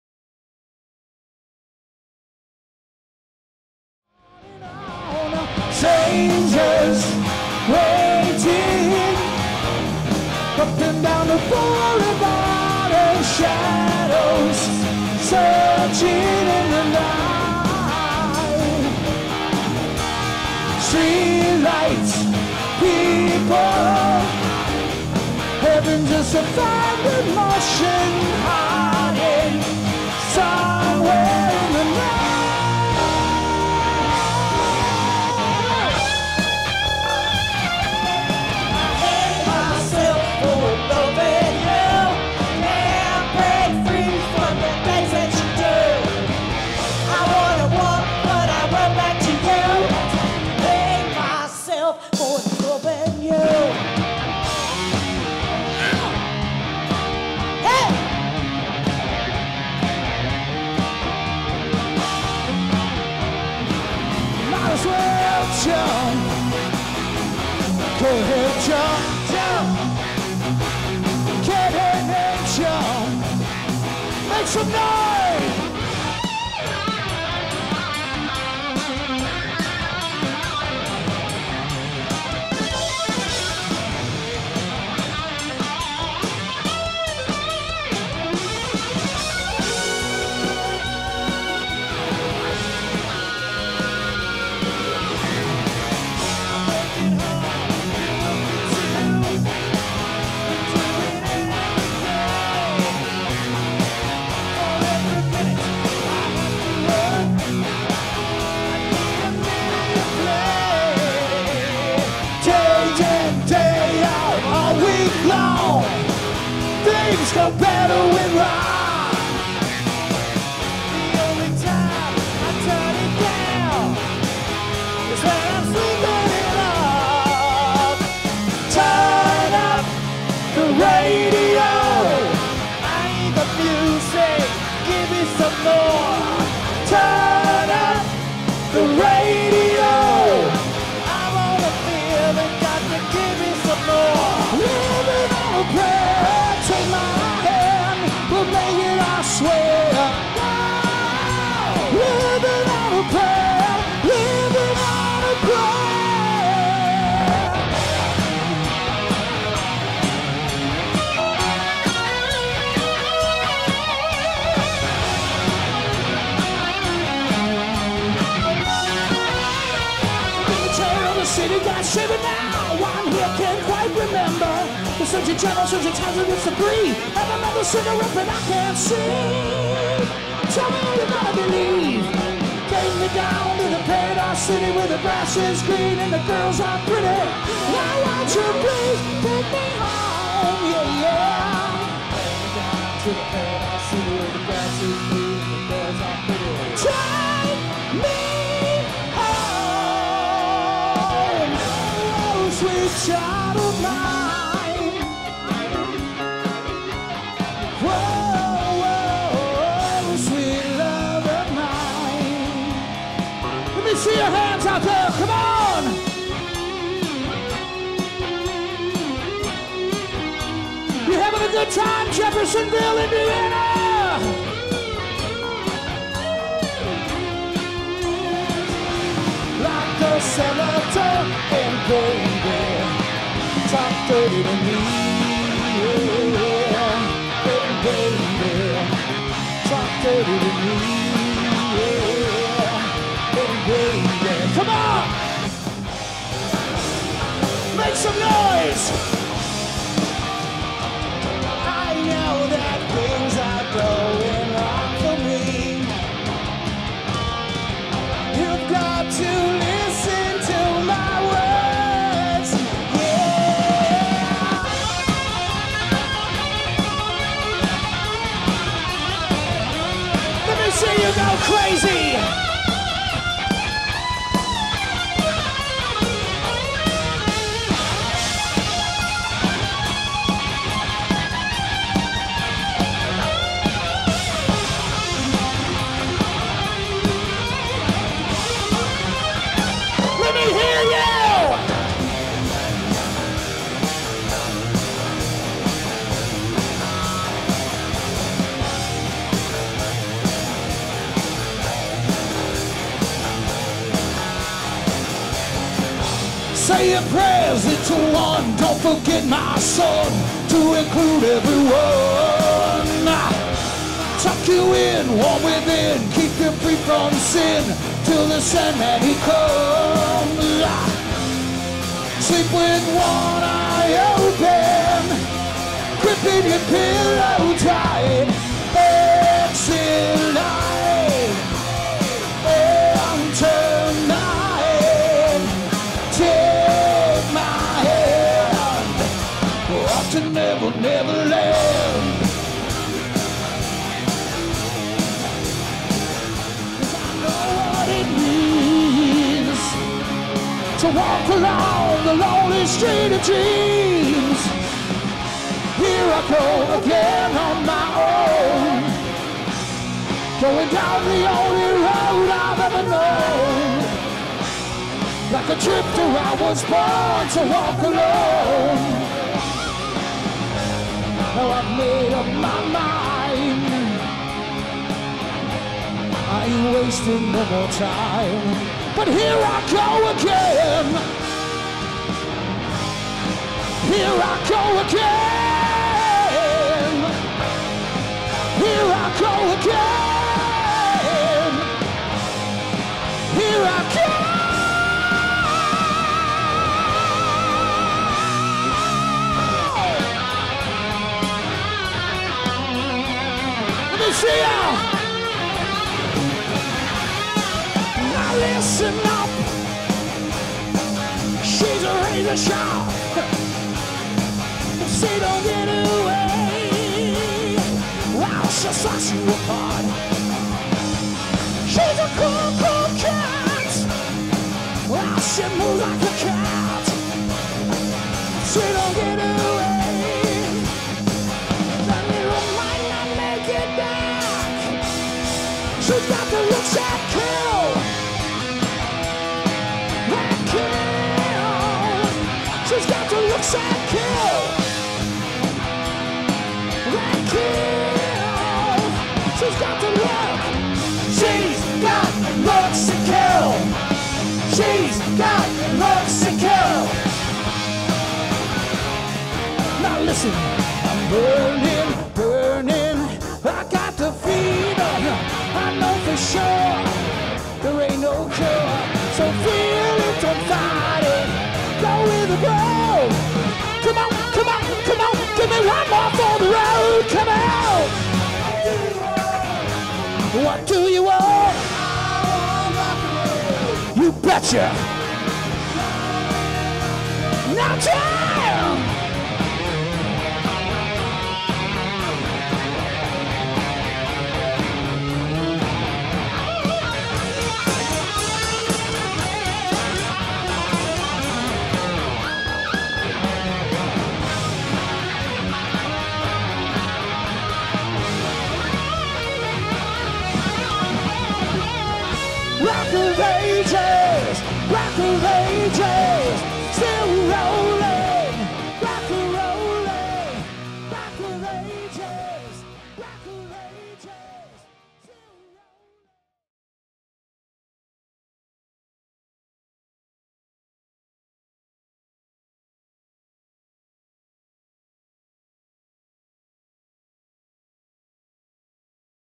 Wicked Sensation Promo (9:00) - Jeff River Stage Concert 7/7/23 - Jeff Parks & Events
@Jeff Parks & Events
Support performance for Thunder Struck